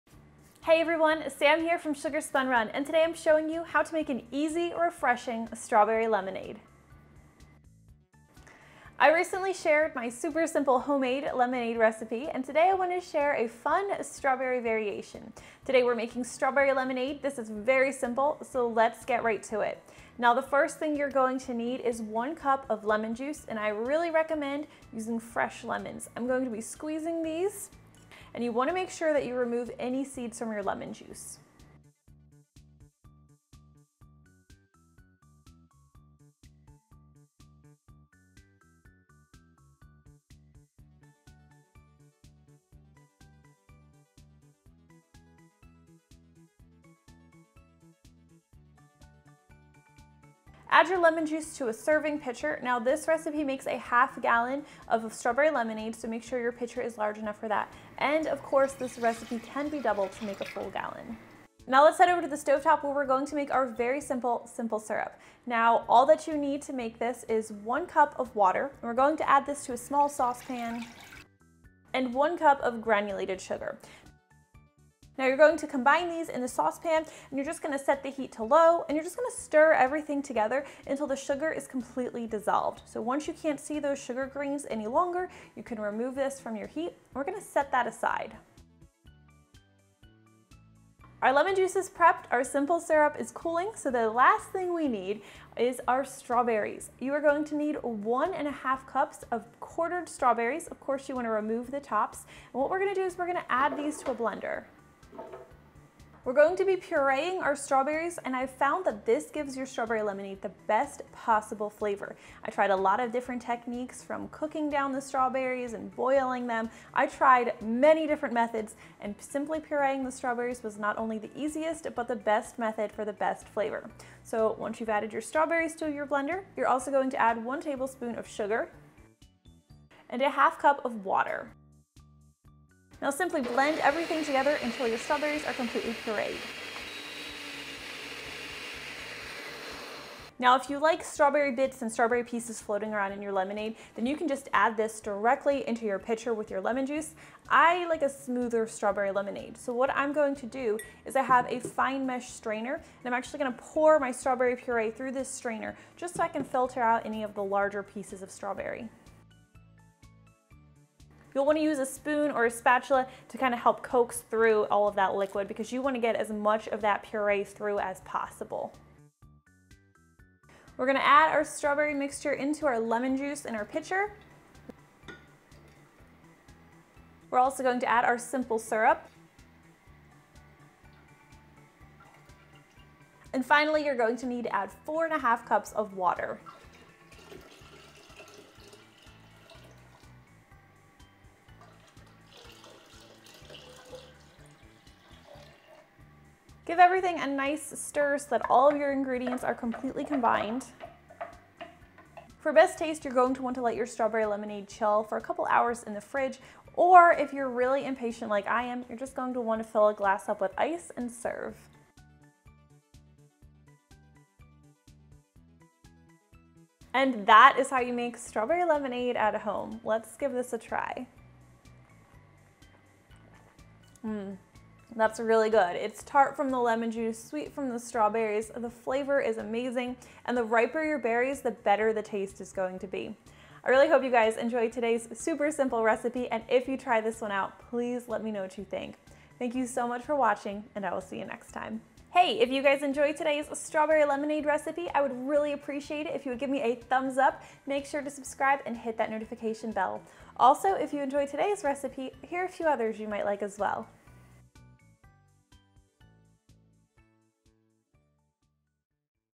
How to Make Strawberry Lemonade
Homemade Strawberry Lemonade isn’t just a perfect refreshing summertime treat, it’s also incredibly simple to make!
↓↓↓↓↓CLICK FOR MORE↓↓↓↓↓↓

This easy recipe is based off of my classic Lemonade recipe and can be prepped with just 4 ingredients in under 10 minutes.

Ingredients
6 cups water divided (1.4 liter)
1 cup + 1 Tablespoon granulated sugar divided (200g + 1 Tablespoon)
1 cup lemon juice¹ seeds removed (236ml)
1 1/2 cups ripe strawberries quartered, plus additional for decoration if desired (210g)

Instructions
00:00 Introduction
01:02 Prepare simple syrup first by combining 1 cup (236ml) water and 1 cup (200g) sugar in a small saucepan. Place over medium/low heat and stir frequently until sugar is dissolved. Set aside.
01:35 Combine strawberries, ½ cup (120ml) of water, and 1 Tablespoon of granulated sugar in blender and blend until strawberries are completely pureed.
02:20 Pour strawberry puree through a fine mesh sieve² into pitcher and use a spatula to press/work as much strawberry juice through as possible. Discard remaining pulp/seeds.
02:57 Add lemon juice and simple syrup to pitcher.
03:03 Add remaining 4 ½ cups (1065ml) water and stir well.
03:23 Pour over ice and serve or chill and then stir again before serving.

Notes
¹Fresh squeeze preferred, but bottled lemon juice will also work
²I like my strawberry lemonade to be nice and smooth and I don’t like pulpy, seed-y bits of strawberry in my lemonade, which is why I use the strainer. If you don’t mind the texture, feel free to skip the straining and leave this in your lemonade!